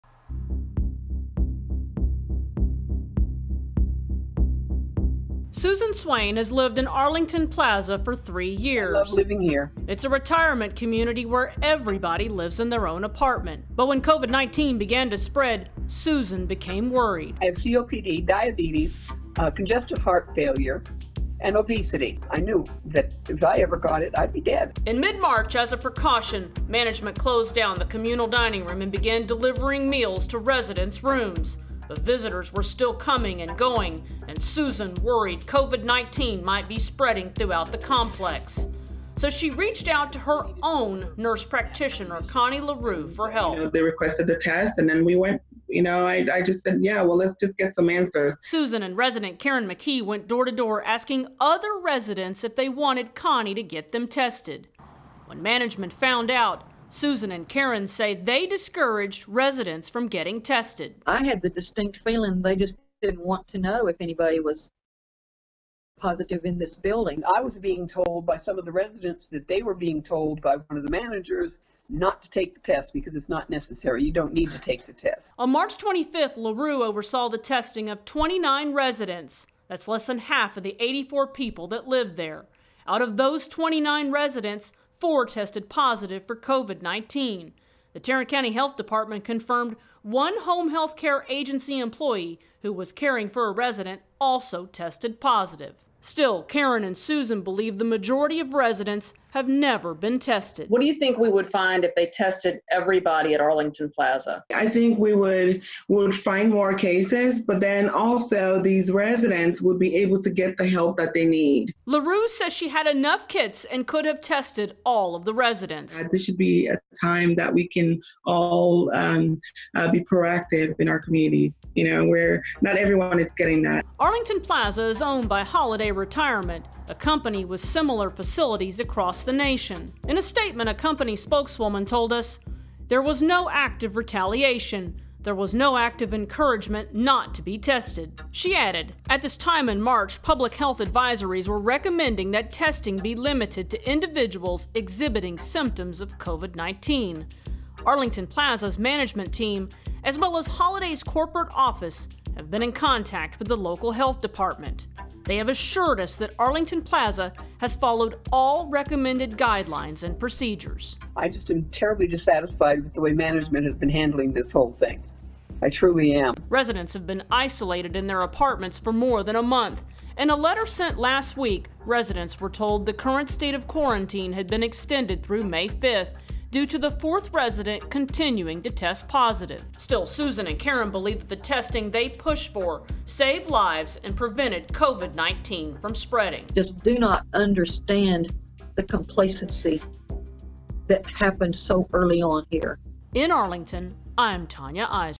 WFAA Investigates: Two residents of Arlington retirement community says management discouraged peopl
“I had the distinct feeling they just didn't want to know if anybody was positive at this building," resident says.